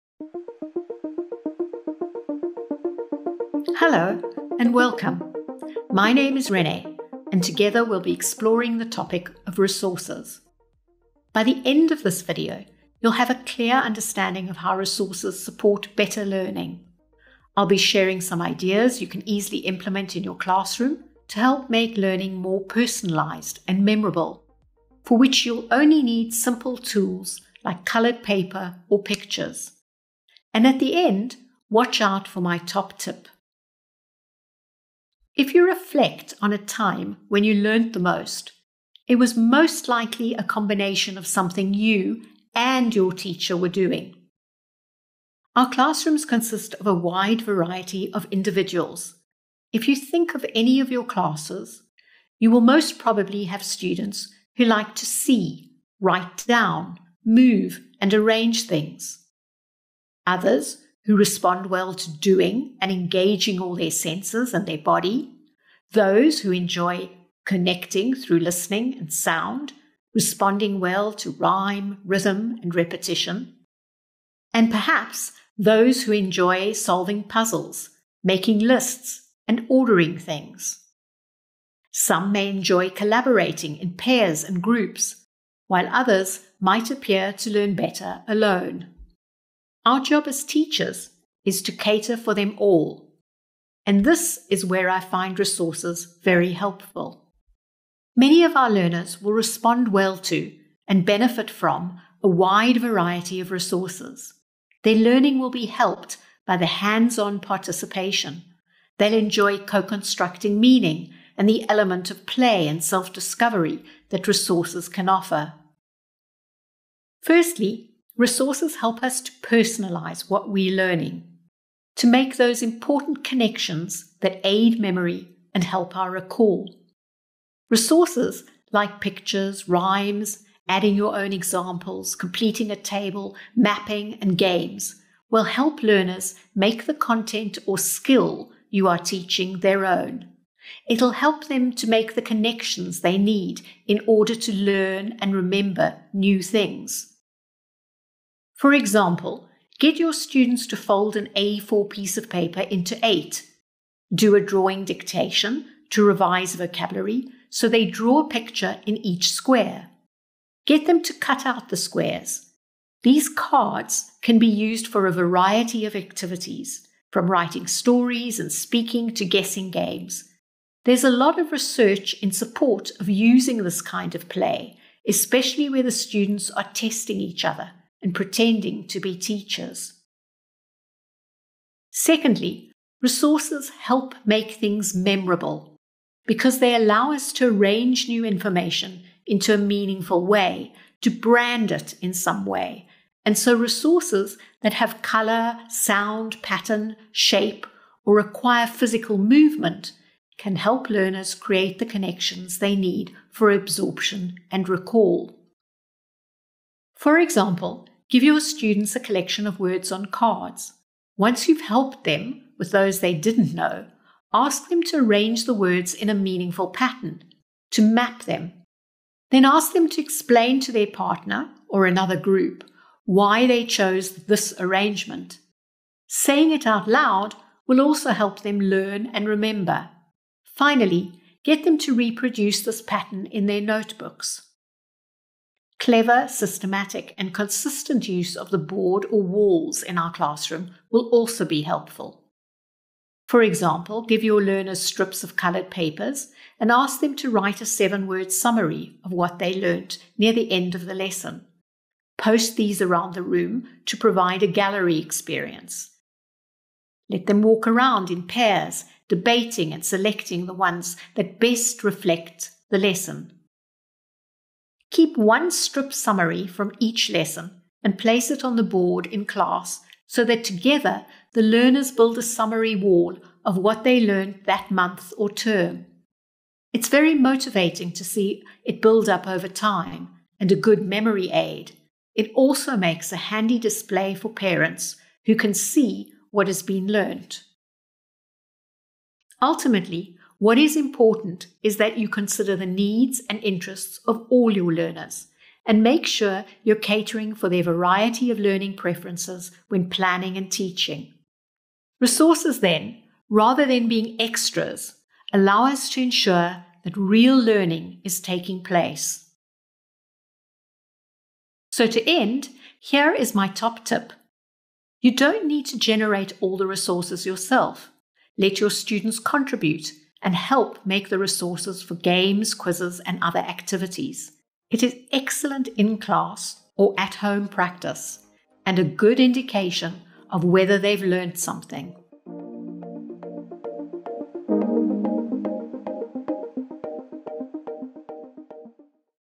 How can resources support learning (Part 3/6)
In this session, Renne Selikowitz discusses how resources can support learning. She also shares ideas of resources you can easily implement in your classroom to make learning more memorable and personalised.

This is part three of six.